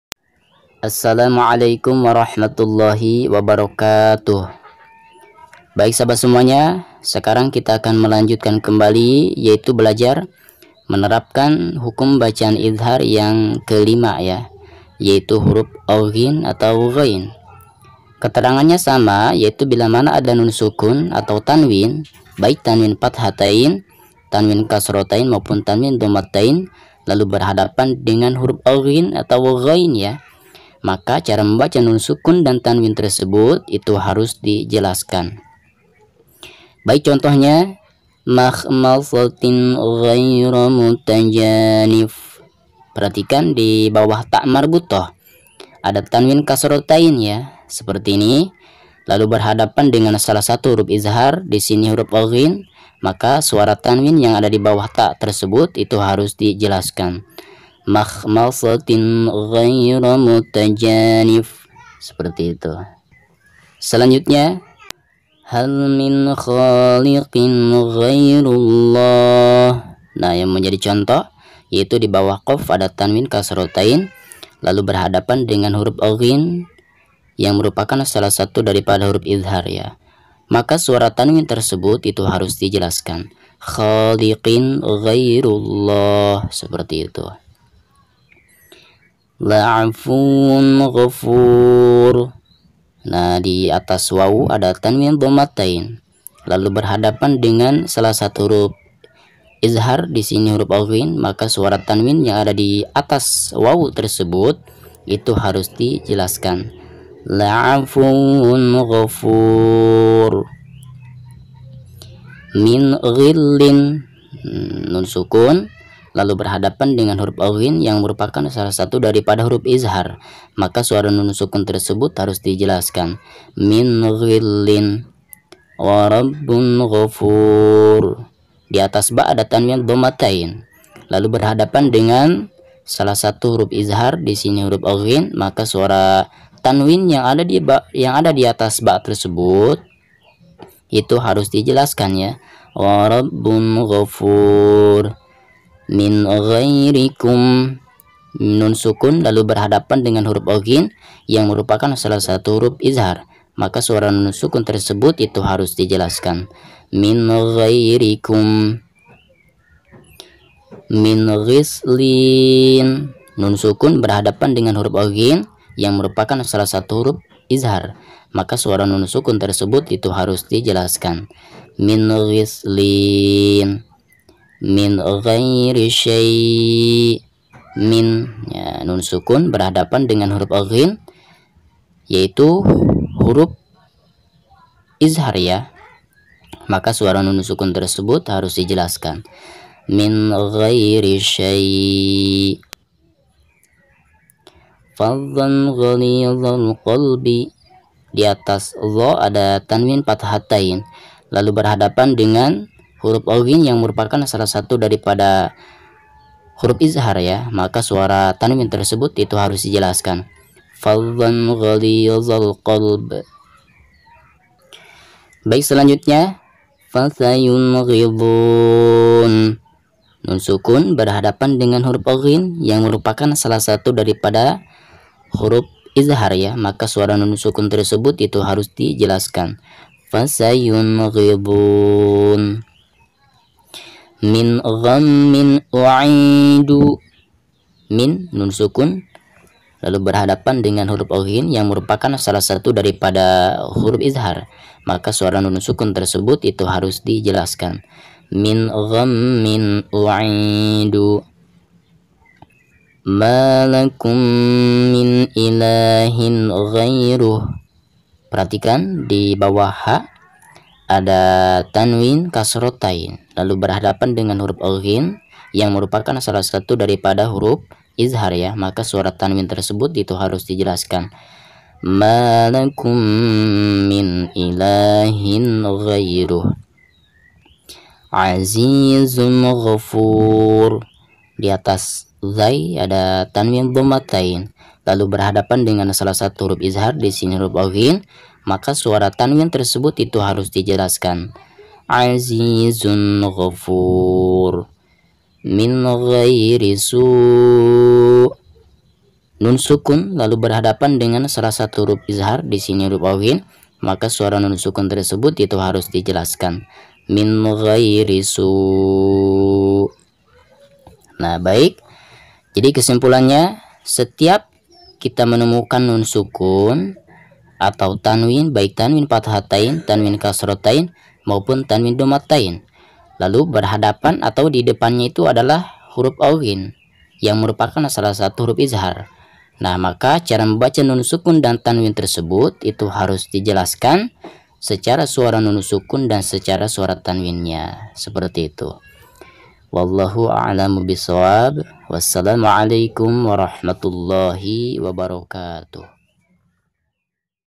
Belajar menerapkan tajwid hukum nun sukun dan tanwin / idzhar halqi hutuf غ
Assalamu'alaikum
kembali lagi bersama pondok iqro
sekarang kita akan melanjutkan belajar tajwid lengkap beserta contohnya idzhar halqi hutuf غ